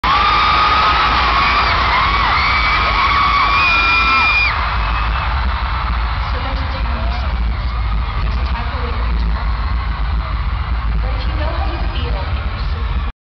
TSwift in Manila: Opening (short clip)
Taylor Swift Live in Manila! for the Speak Now World Tour 2011